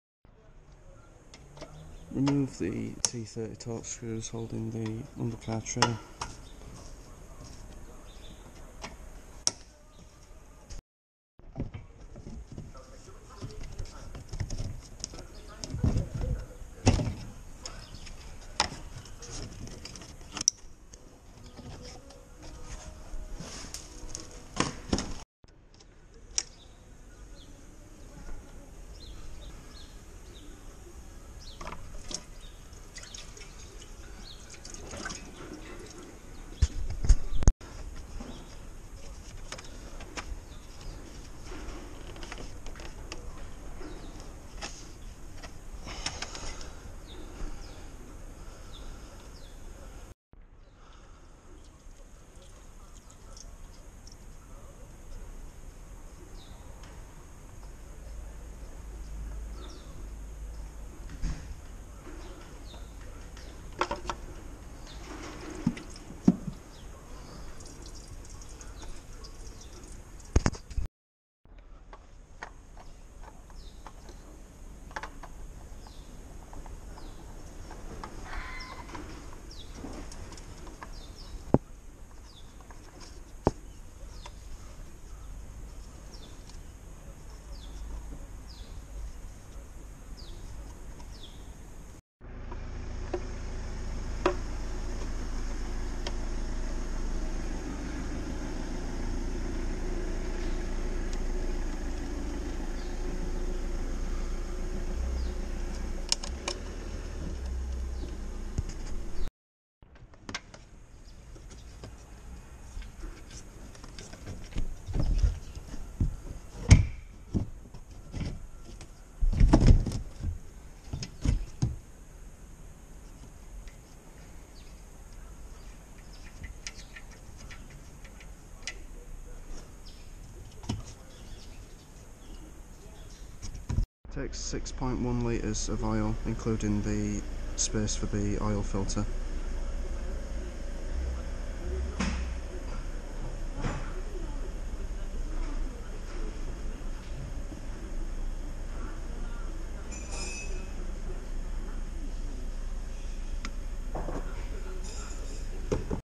Ford Mondeo MK5 2.0 TDCi Oil and Filter Change (Peugeot/Citroen engine)
How to change the oil and filter on the MK5 Ford Mondeo 2.0 TDCi